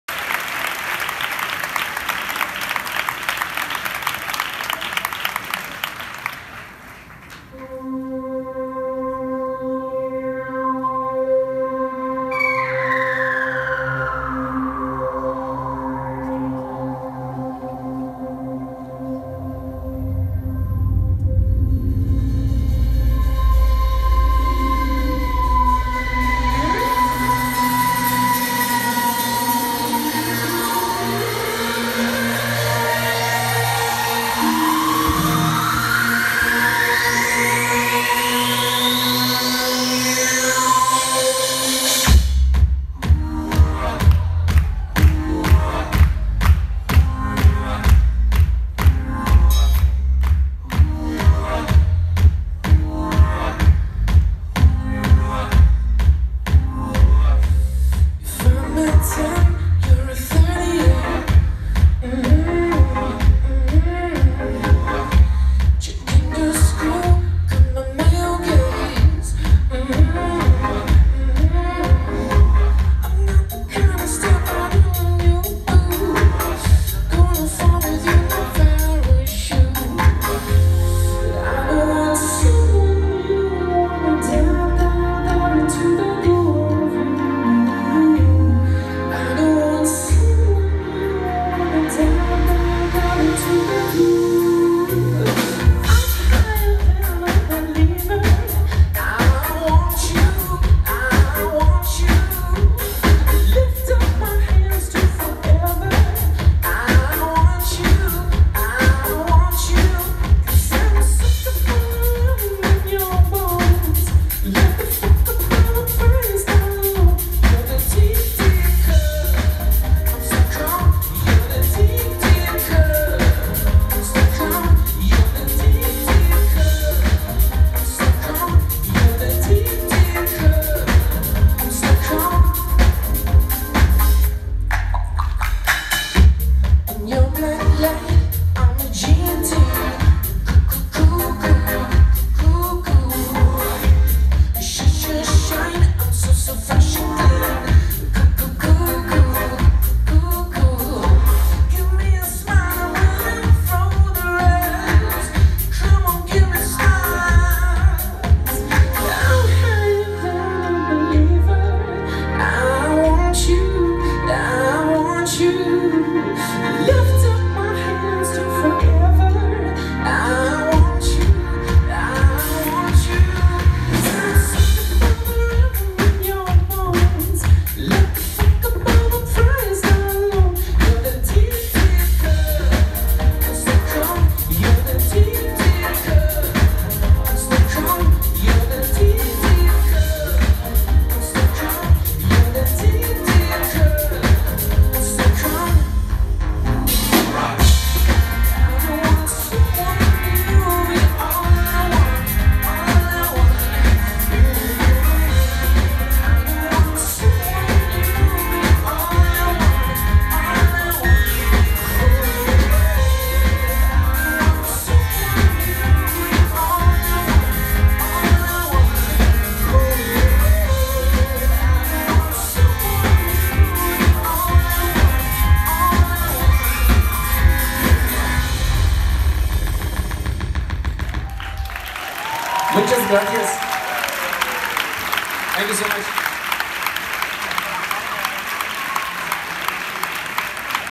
Electro Pop Concierto 2019
Gala Alan Turing LGTBIQ Awards presentada por Rosy de Palma y Jose Lamuno y como invitado a entrega de premios Boris Izaguirre, en Tenerife, Canary Islands.
LGTBIQ (Lesbianas, Gays, Bisexual, Trans, Intersex y Queer).